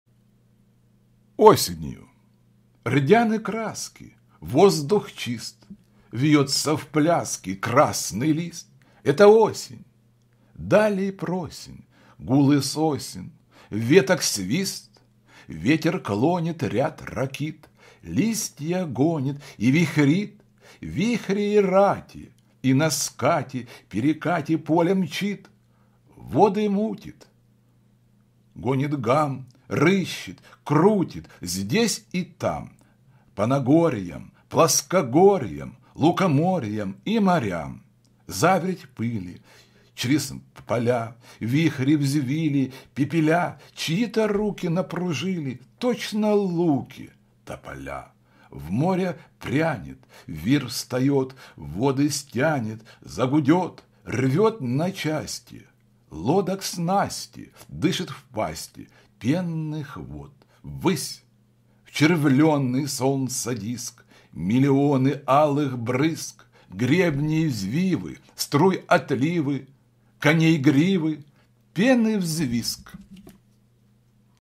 Осенью ,Максимилиан Волошин ,читает Павел Беседин
Рдяны краски,
Воздух чист;
Вьётся в пляске
Красный лист, —
Это осень,
Далей просинь,
Гулы сосен,
Веток свист.

Ветер клонит
Ряд ракит,
Листья гонит
И вихрит
Вихрей рати,
И на скате
Перекати-
Поле мчит.

Воды мутит,
Гомит гам,
Рыщет, крутит
Здесь и там —
По нагорьям,
Плоскогорьям,
Лукоморьям
И морям.

Заверть пыли
Чрез поля
Вихри взвили,
Пепеля;
Чьи-то руки
Напружили,
Точно луки,
Тополя.

В море прянет —
Вир встаёт,
Воды стянет,
Загудёт,
Рвёт на части
Лодок снасти,
Дышит в пасти
Пенных вод.

Ввысь, в червлёный
Солнца диск —
Миллионы
Алых брызг!
Гребней взвивы,
Струй отливы,
Коней гривы,
Пены взвизг…